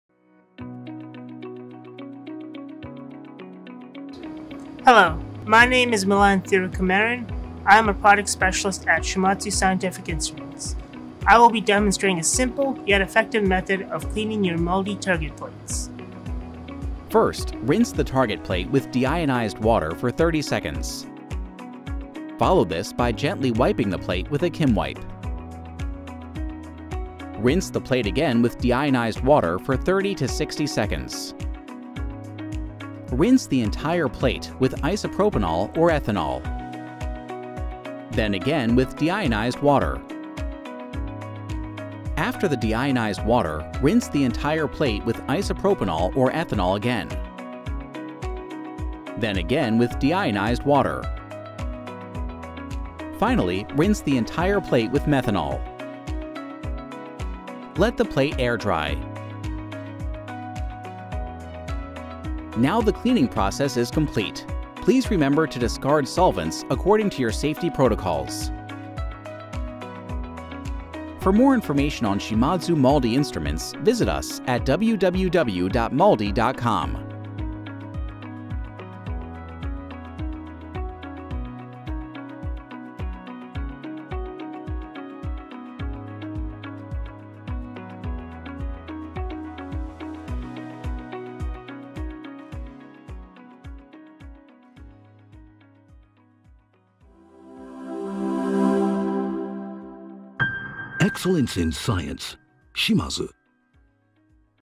How To Clean a MALDI Target Plate
This video demonstrates a simple yet effective method to clean MALDI target plates.